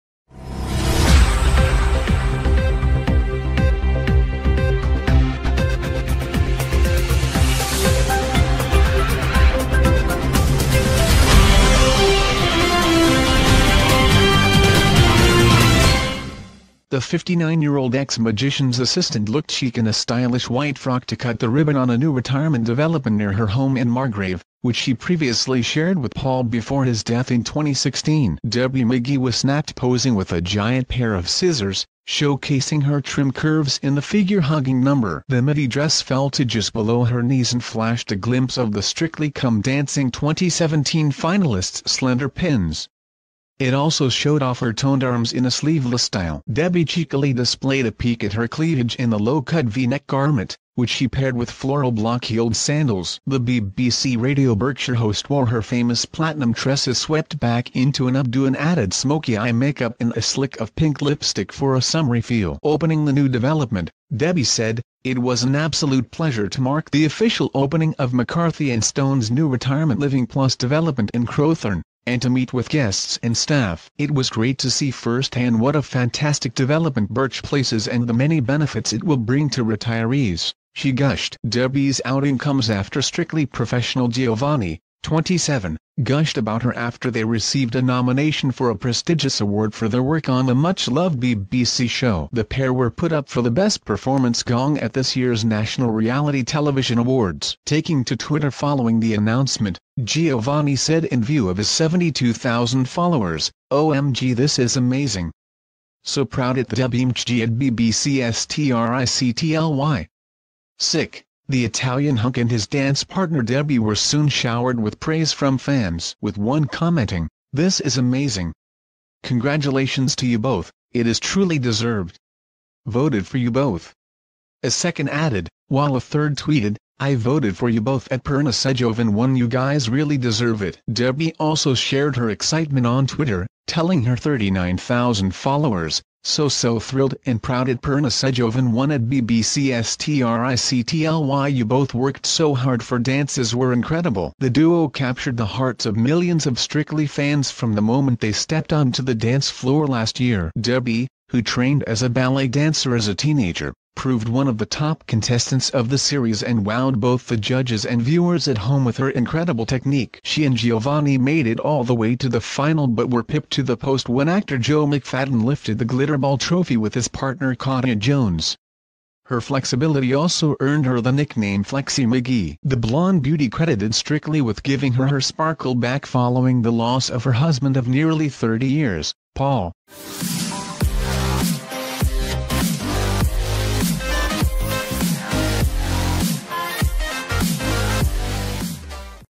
Debbie McGee: Paul Daniels' Strictly star wife stuns in plunging frock after Giovanni news
The 59-year-old ex-magician’s assistant looked chic in a stylish white frock to cut the ribbon on a new retirement development near her home in Margrave, which she previously shared with Paul before h